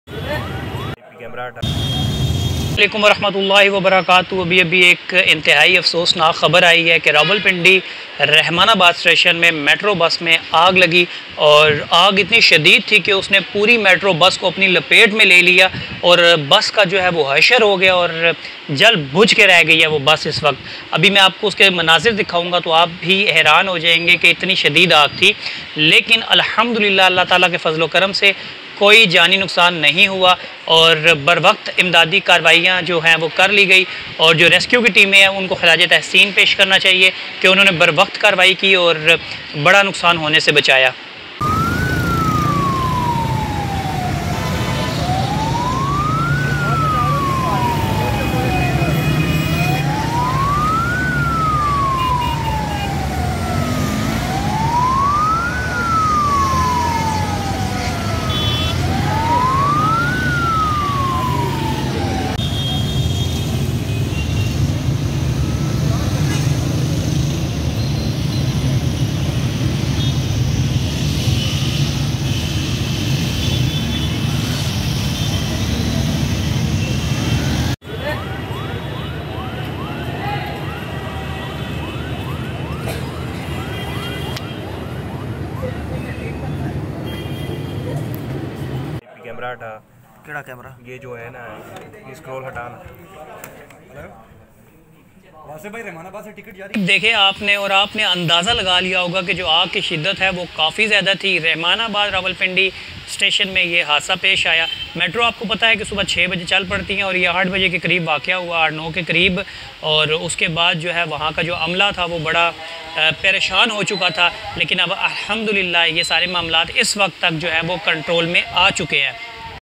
Rawalpindi Rehman Abad station Metro bus Mai Aag Lag Gayi , Chaudhry Qasim
Rawalpindi metro station Mai bus Mai Aag Lag Gayi